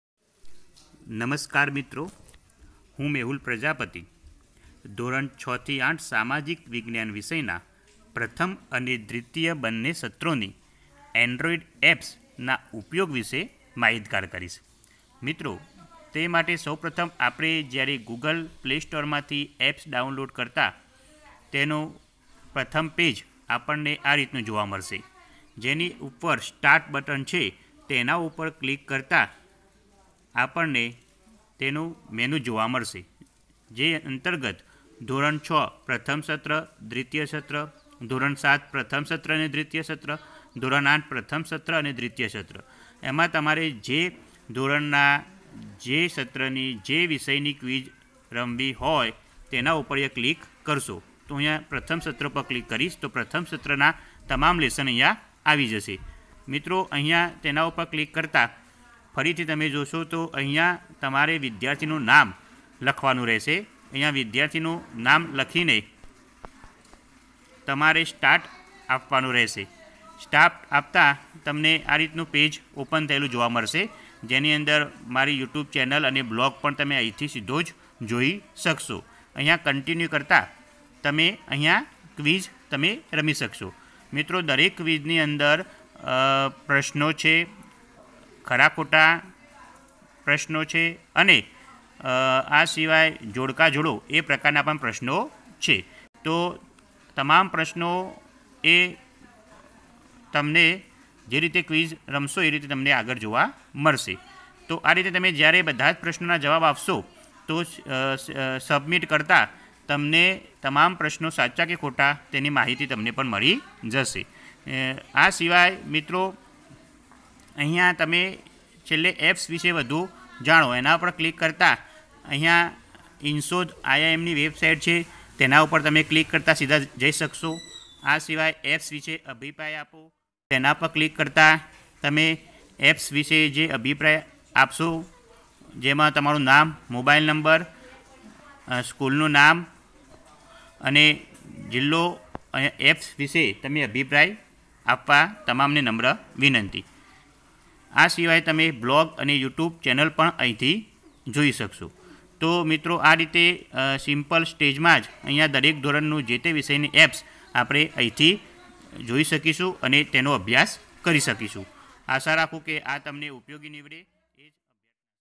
SOCIAL SCIENCE STD 6 TO 8 APP INTRO BY MEHUL PRAJAPATI
સામાજિક વિજ્ઞાન ધોરણ:-૬ થી ૮ પ્રથમ અને દ્વિતીય સત્રના નવા(NCERT) અભ્યાસક્રમ મુજબની ઓનલાઇન ક્વિઝ
Social Science New Syllabus (NCERT) Std-6 to 8 First & Second sem Online Quiz – No Ads In this Apps
Mehul prajapati presents the gujarati quiz android app for Social science Subjects of std- 6 to 8 First & second sem.Social Science Quiz is the best way to improve your knowledge of social science.This app has been developed for testing your skills of Social science.Star Features of the app are given below
App Features:-
- Simple Gujarati language App
- This is an online android app with low size mb only.
- You can play chapter wise quiz of class 6 to 8 Social Science First & Second Sem.
- There are 1200+ question included in this Social science quiz and more questions will be   added in future without App Update…
- You can see all right and wronge answer and of the Quiz
- There is not any disturbance of Advertisement in this App
- This is very useful app for upper primary school students,teachers and others also
- This app can be used for exercise & evaluation
- Also useful for competitive exams like TET,t, ,UPSC,NMMS,And Others…